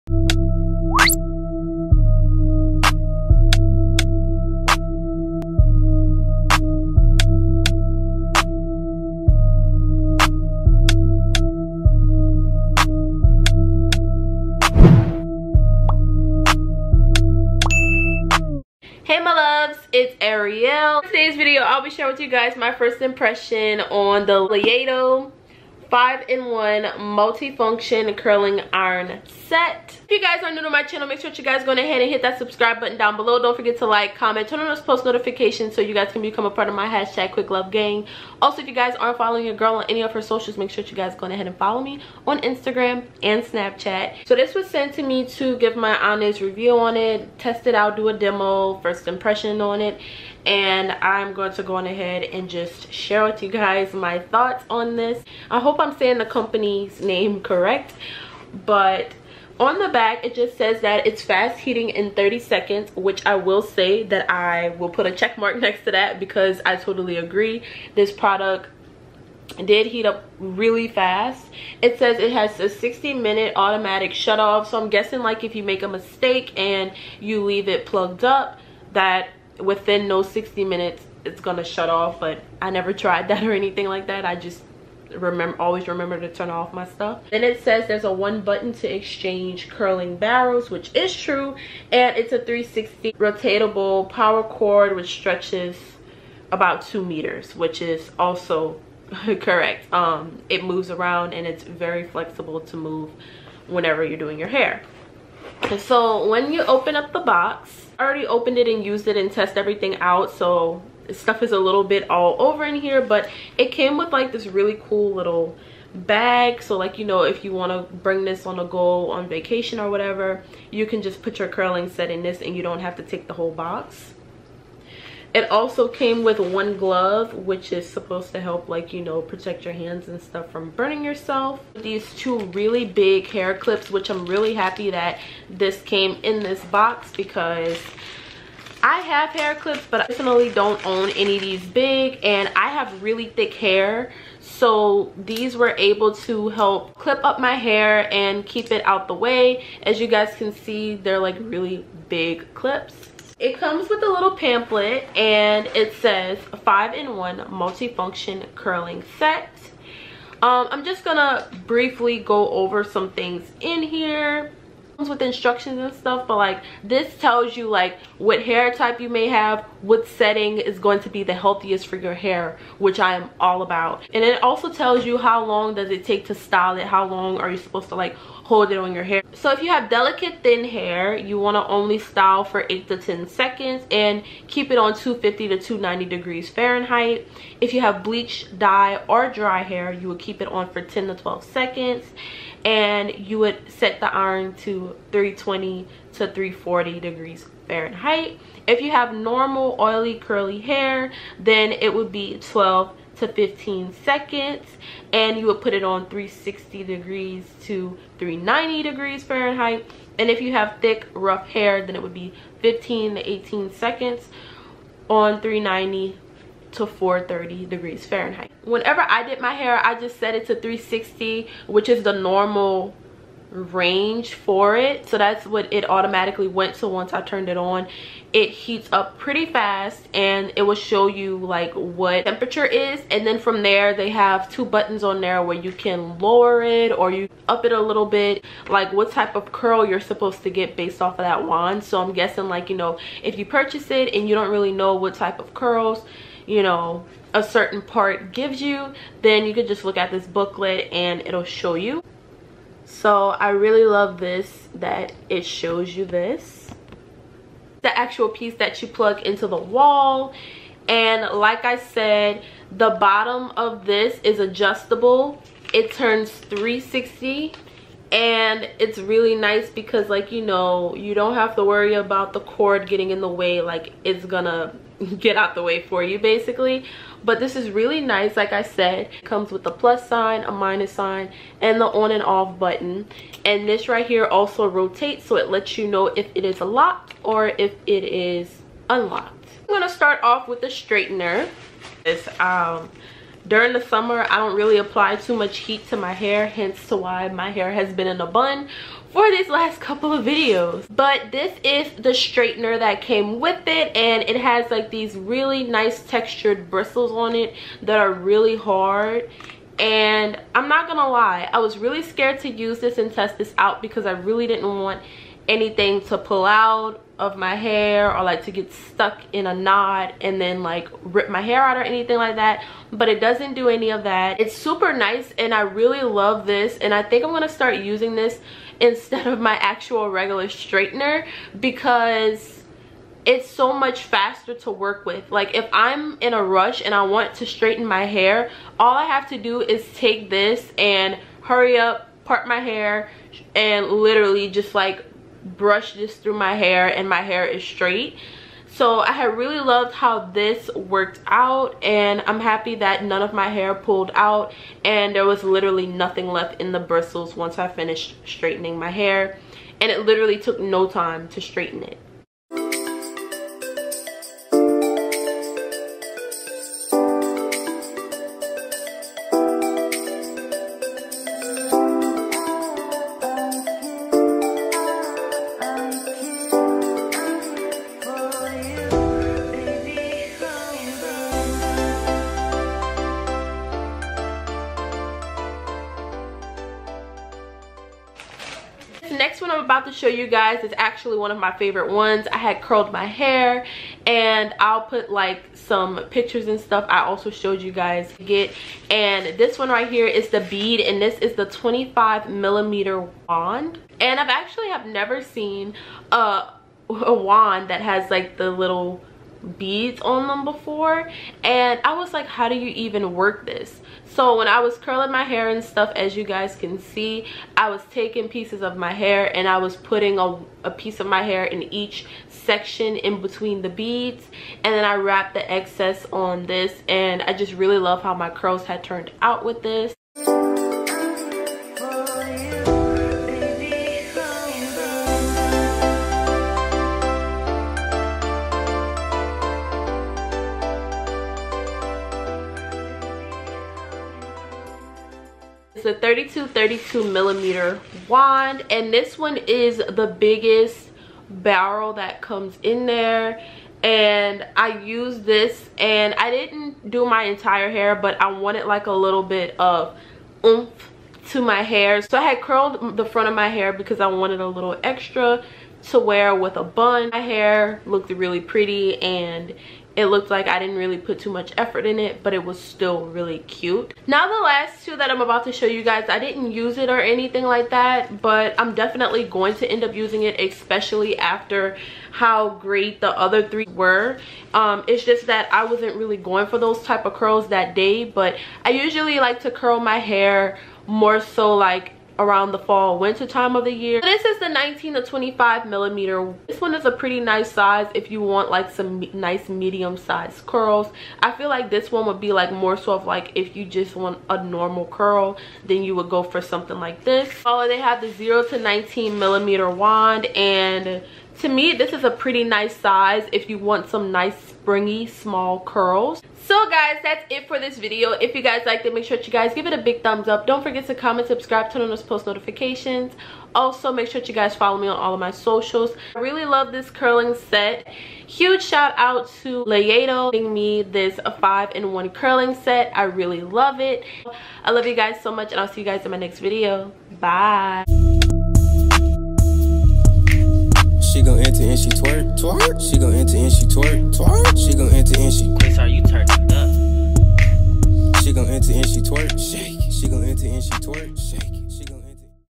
LAYADO 5 IN 1 CURLING IRON | AMAZON REVIEW
Hey my loves, today I’m coming to you guys with a bonus video. For today’s video I was sent this 5 in 1 curling set to test and review from layado.

Questions:
What’s your name? Eriel
Where are you from? Louisiana
Random facts: I love wingstop, iced coffee, bath products, makeup & glue on nails lmao!

Film/Edit:
iMovie
Videoleap